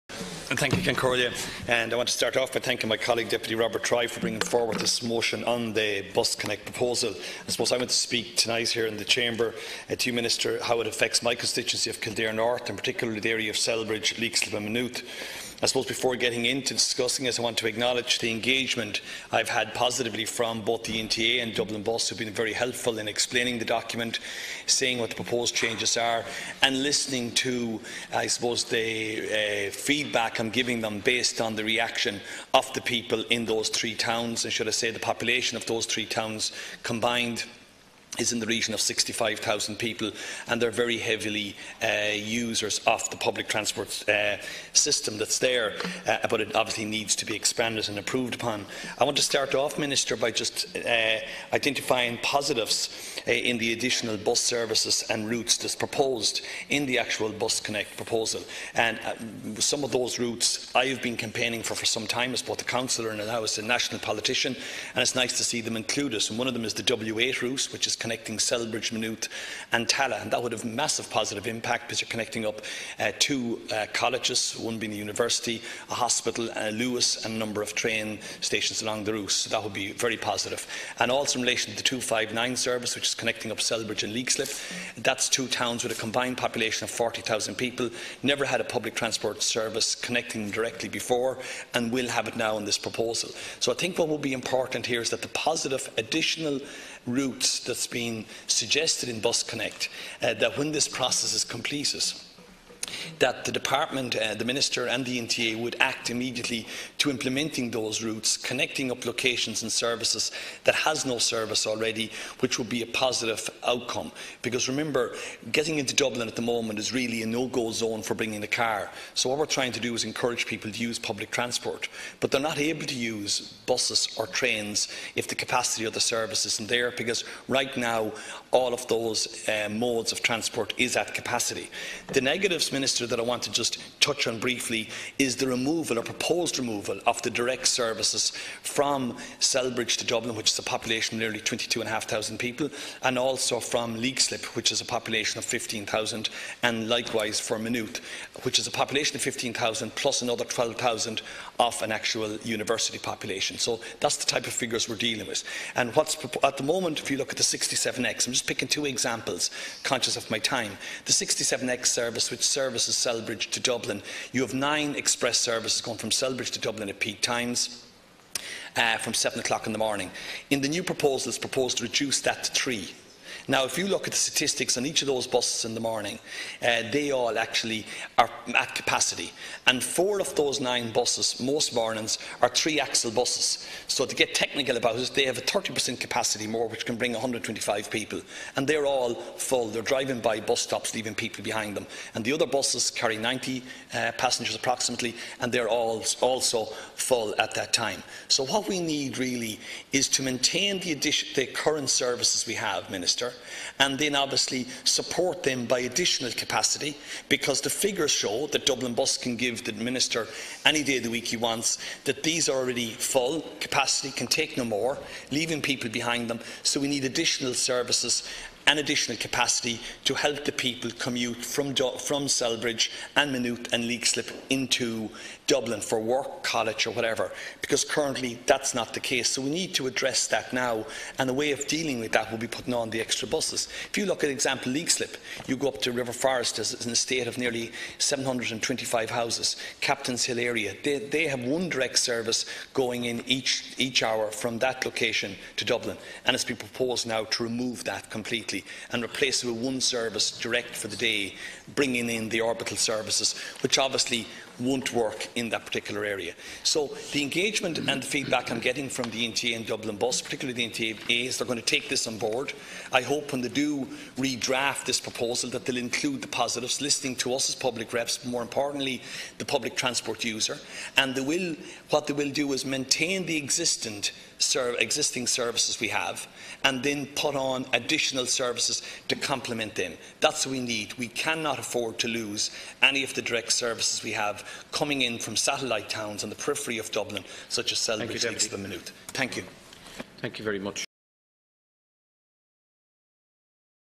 Deputy Frank O'Rourke - BusConnects: Motion - 19.09.2018
Dáil Éireann debate - Wednesday, 19 Sep 2018
BusConnects: Motion [Private Members]

Are you interested in politics? Are you looking for a speech by your TD?